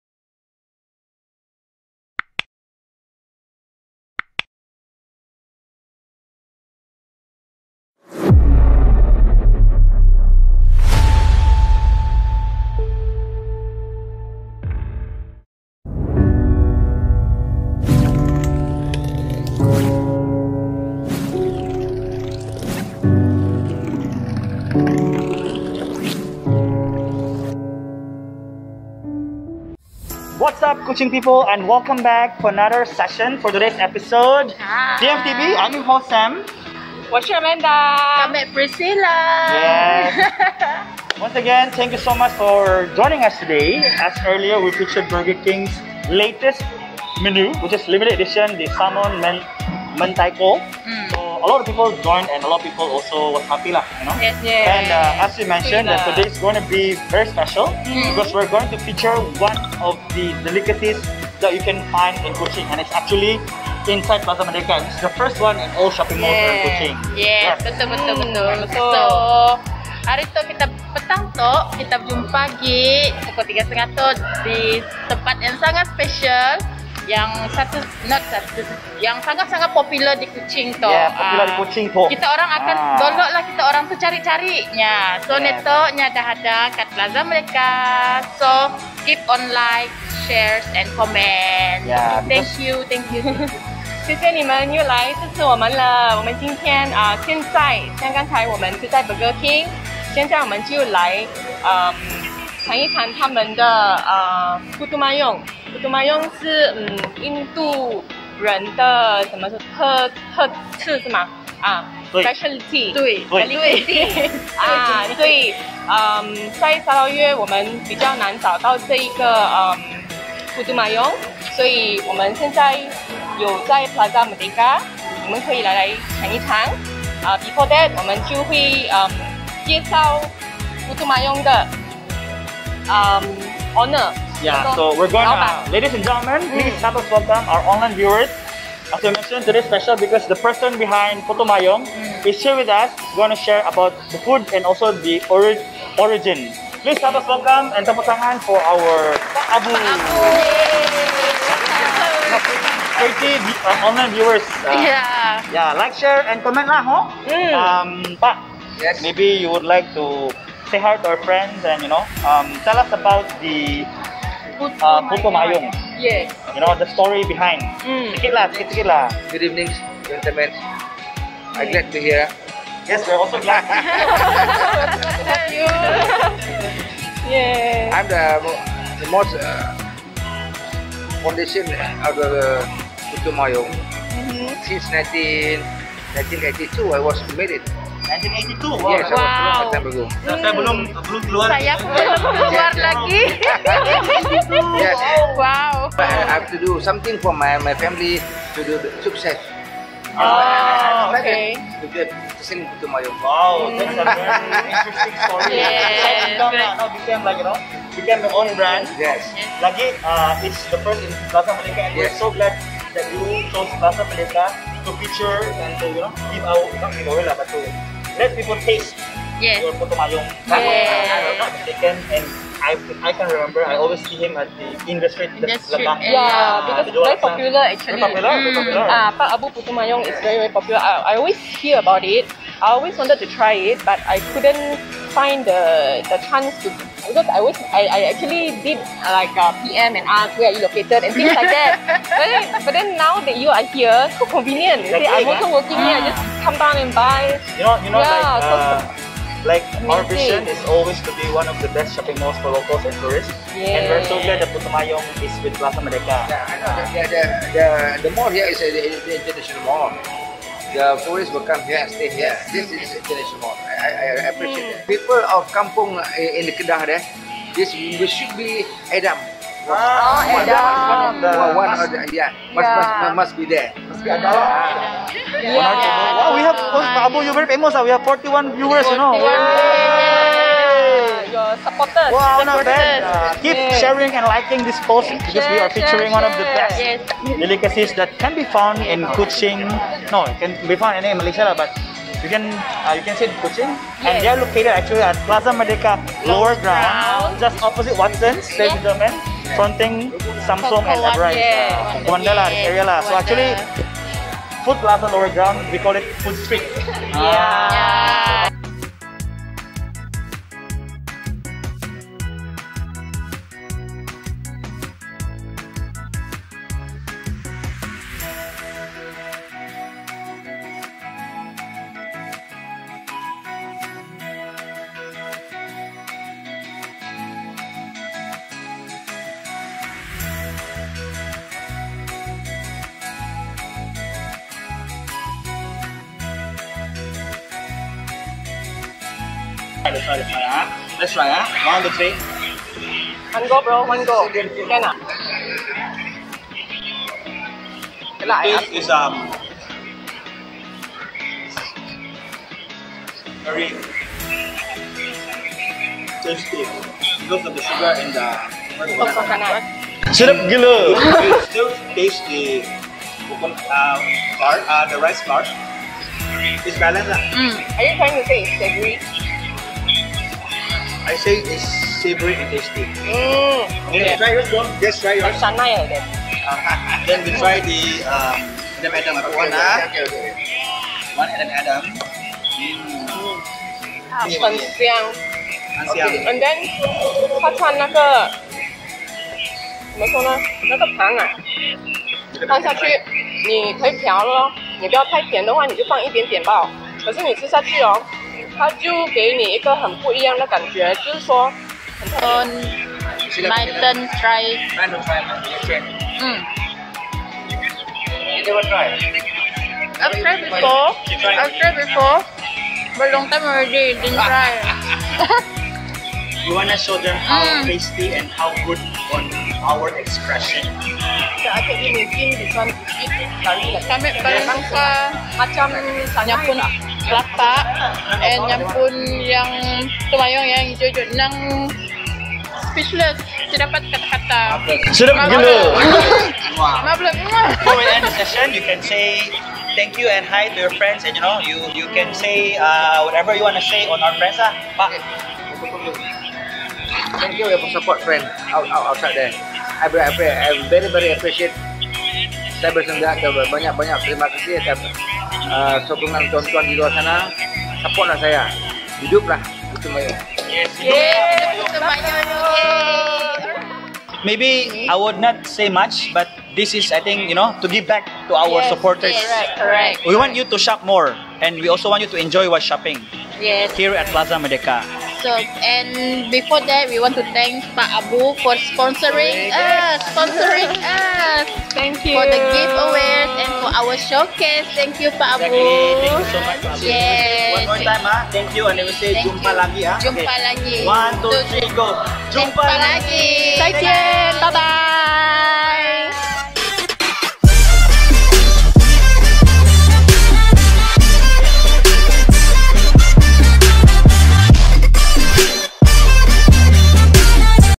PMTV meets PAK ABU PUTU MAYUNG
Started off on a motorbike, this Iconic Street food is now available at Plaza Merdeka. PMTV team meets Pak Abu and had a first hand Putu Mayung experience.